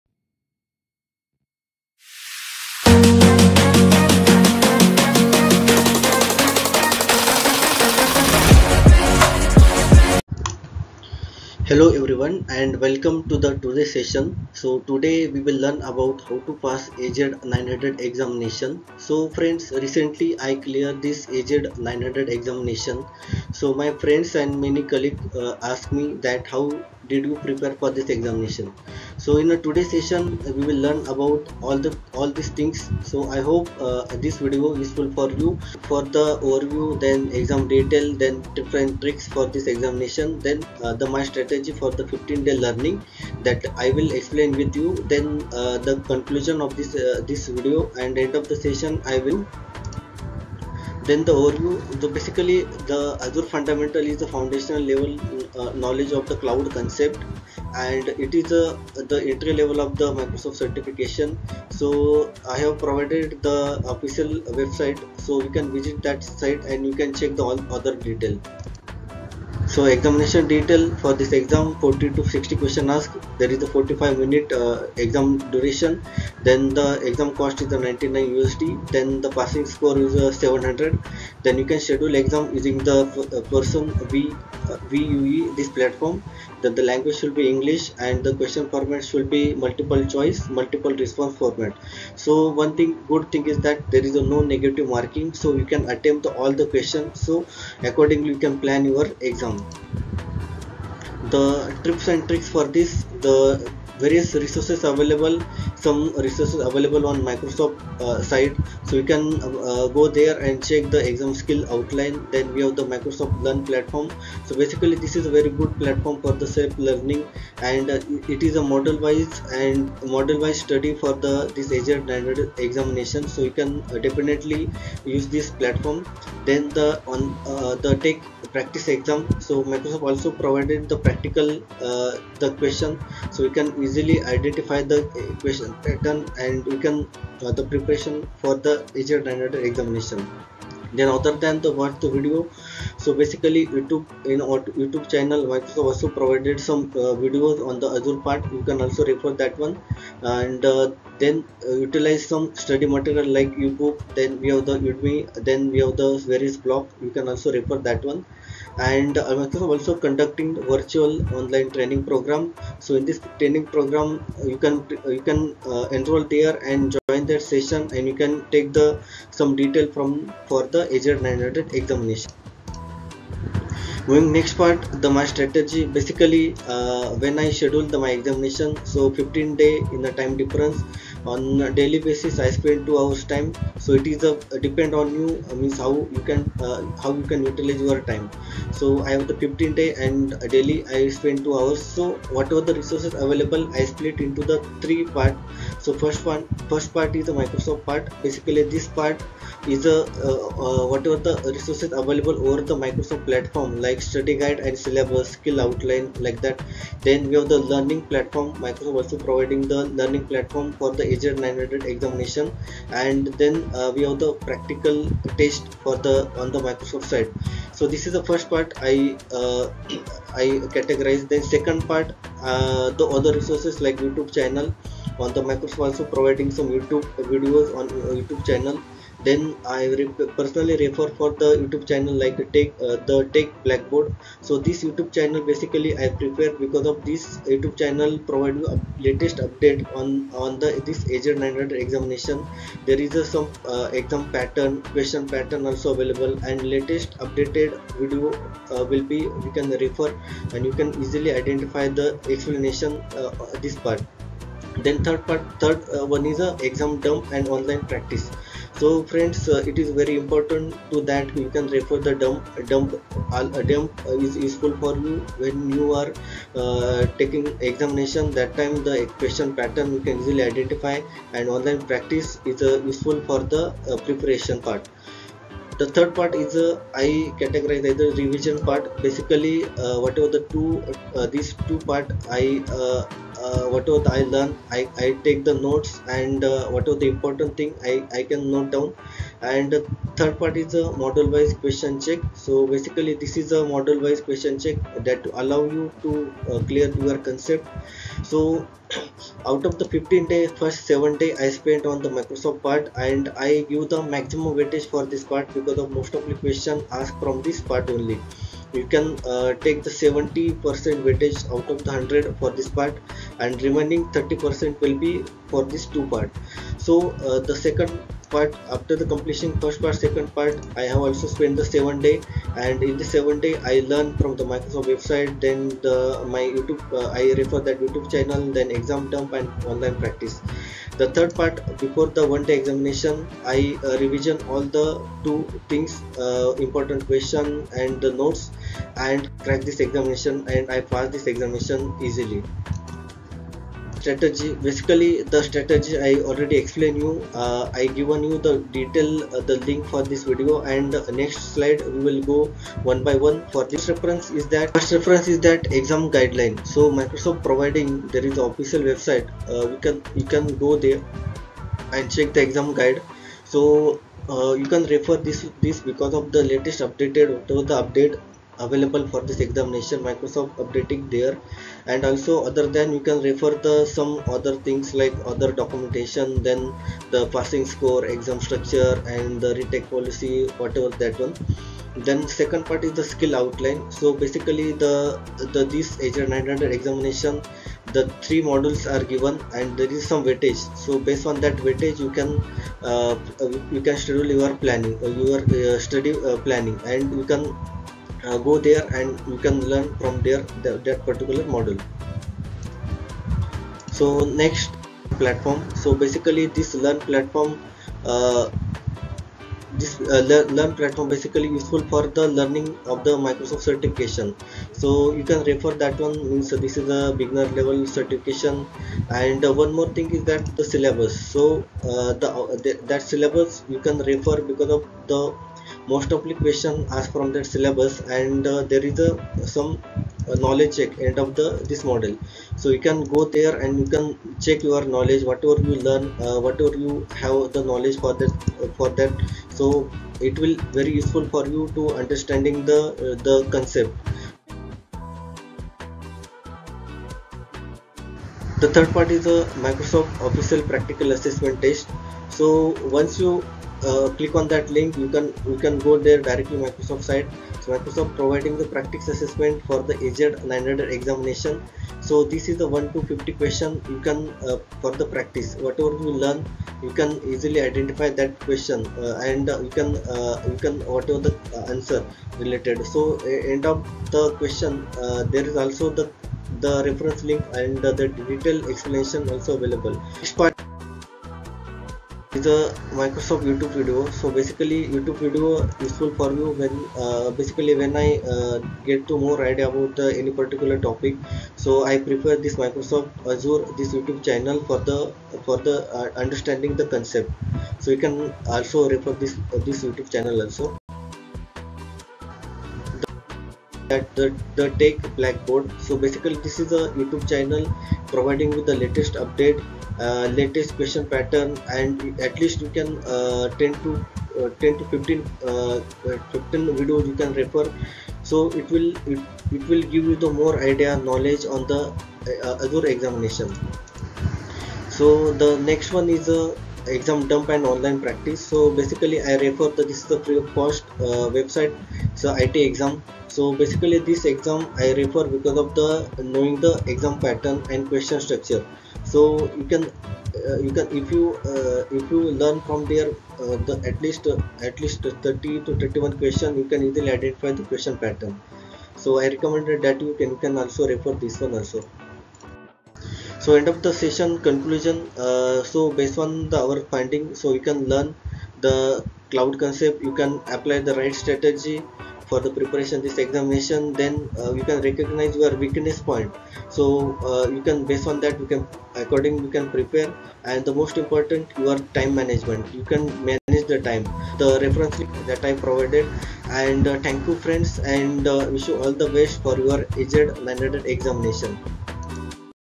How to Pass Azure Fundamentals AZ-900 Certification in 15 Days | Preparation Strategy for AZ-900
Hello there! This video is an overview of how I pass Microsoft Azure Fundamentals Certification! The AZ-900 exam is a certification that validates the foundation-level Azure cloud skills and knowledge that you need as an IT Professional.
In this Video i explain details Preparation Strategy i follow, and my experience about the certification.

I hope this video helpful for you, and i wish you for AZ-900 Examination.

Thanks,
Pravin Auti.

type=practice&assessmentId=23&practice-assessment-type=certification

Best video for AZ 900 exam passing.
Study guide for exam  AZ -900 : Microsoft Azure Fundamentals